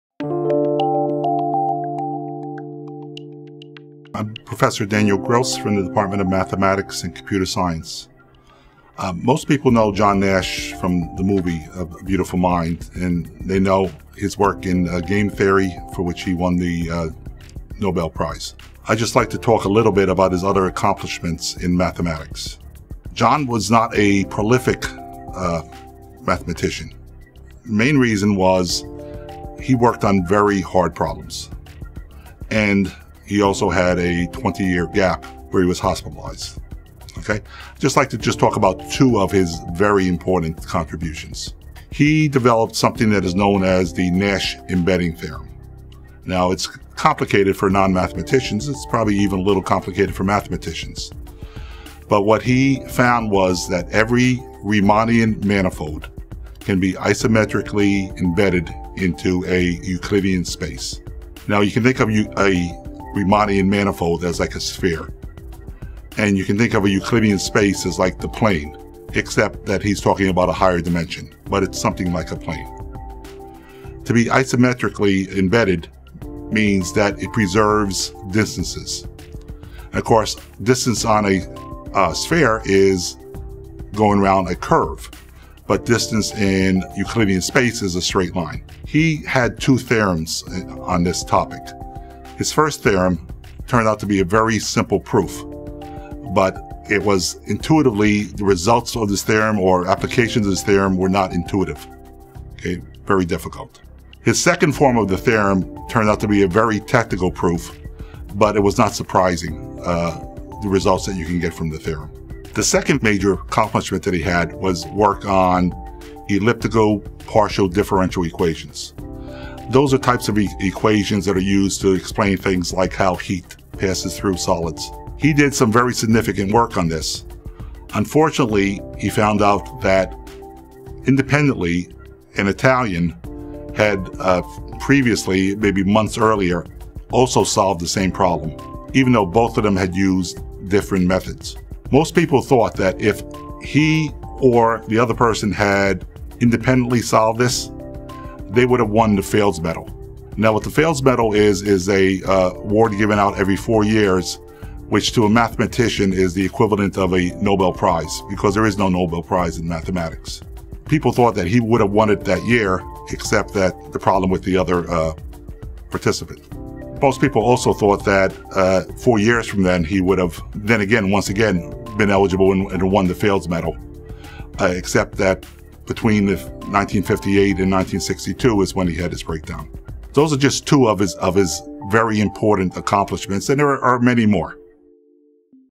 Reflections on John Nash with Daniel Gross
Professor Daniel Gross, Ph.D. speaks about John Nash's contributions to mathematics beyond Game theory, including Nash embedding theorem.